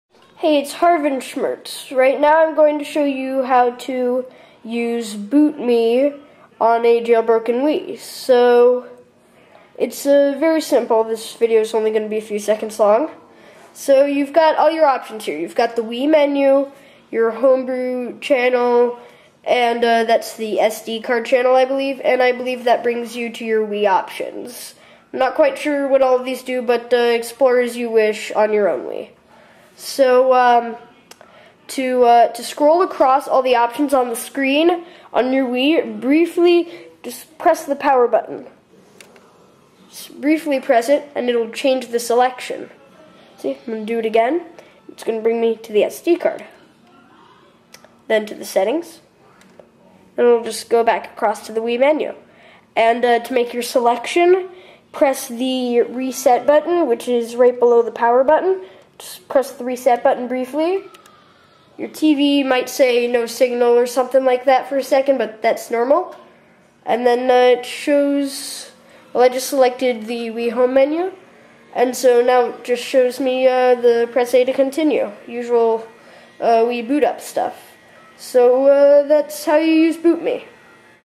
BootMii tutorial
Learn how to use bootmii on a jailbroken wii.